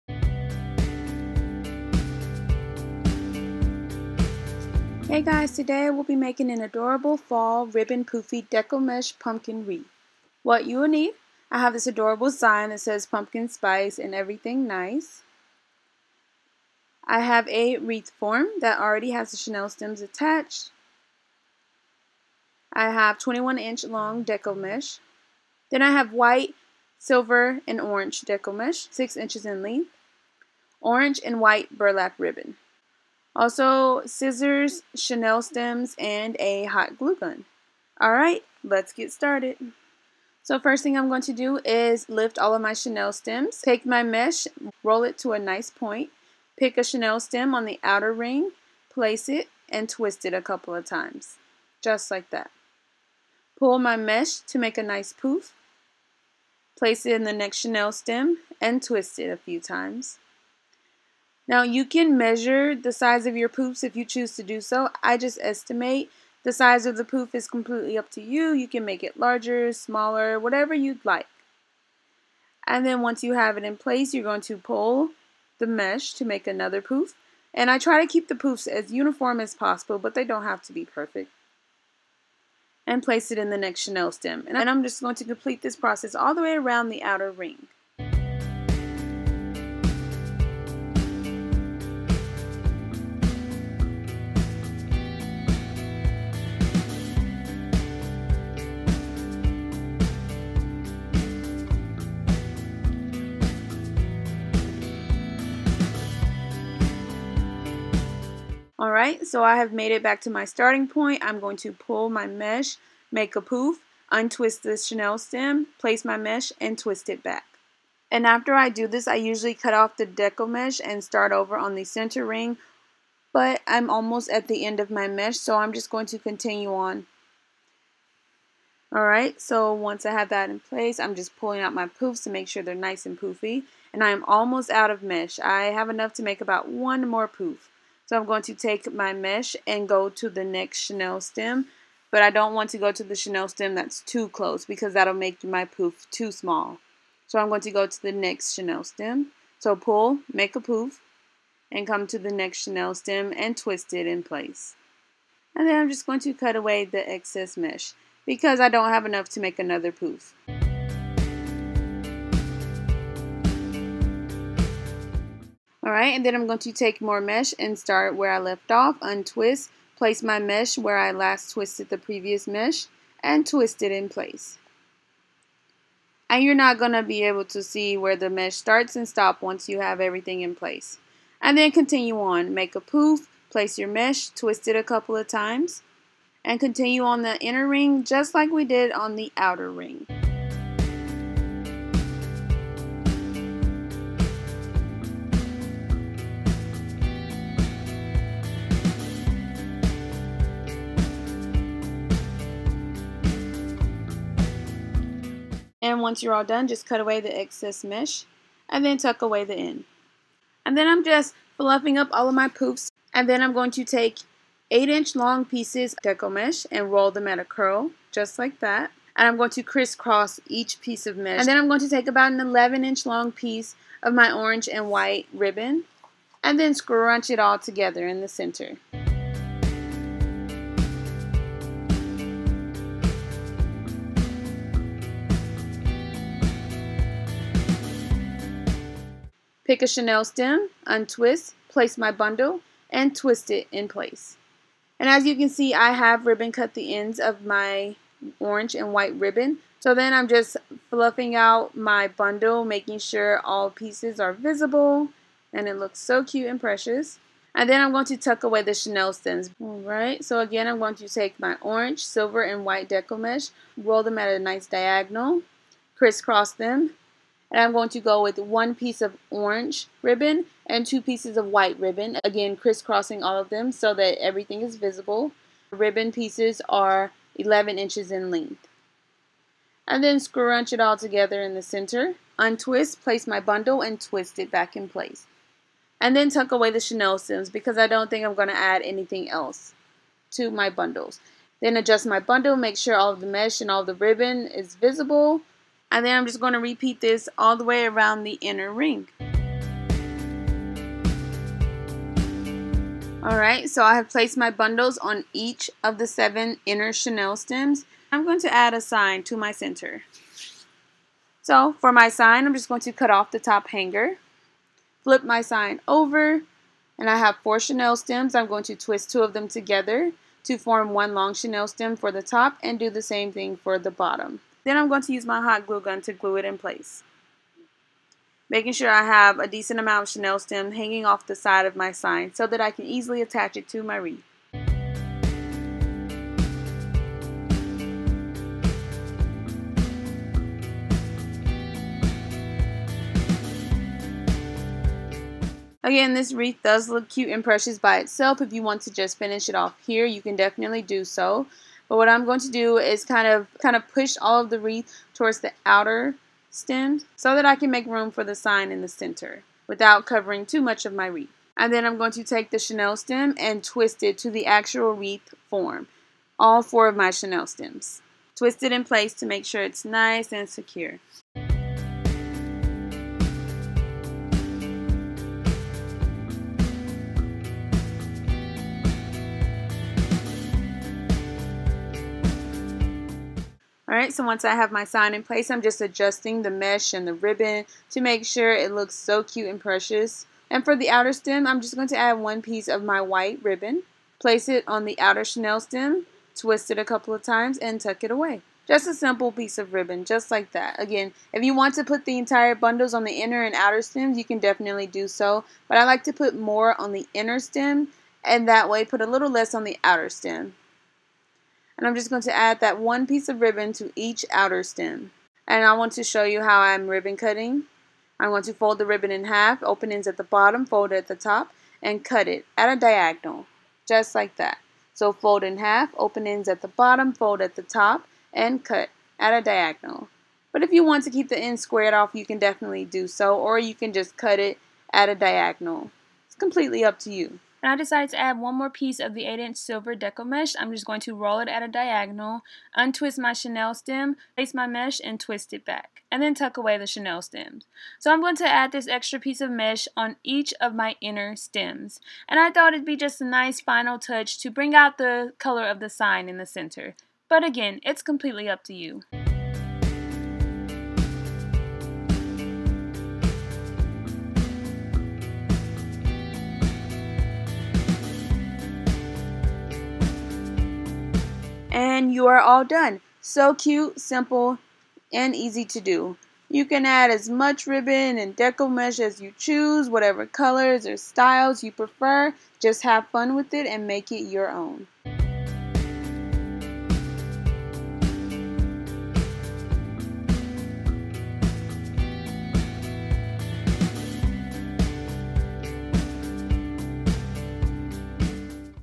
DIY: Fall Ribbon Poofy Deco Mesh Pumpkin Wreath || Quick and Easy
This adorable poofy deco mesh wreath is perfect for fall. Simple and easy to make. Personalize it to fit you! Have fun with it and make it your own!